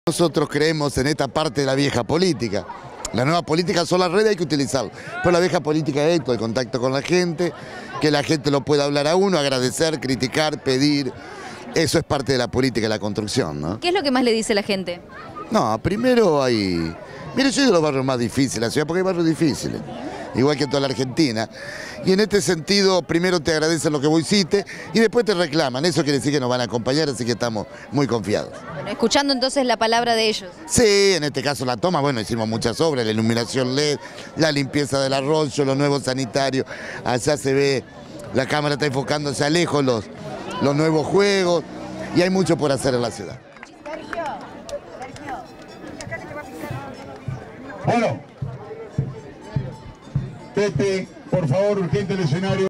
VARISCO
VARISCO VISITÓ PUERTO SÁNCHEZ Y DIALOGÓ CON LOS VECINOS
EL INTENDENTE SERGIO VARISCO, COMENTÓ QUE ESTUVO DIALOGANDO CON VECINOS Y RECORRIENDO LA ZONA. "CREEMOS EN LA VIEJA POLÍTICA, EN EL CONTACTO CON LA GENTE, QUE PUEDAN AGRADECER, PEDIR O CRITICAR",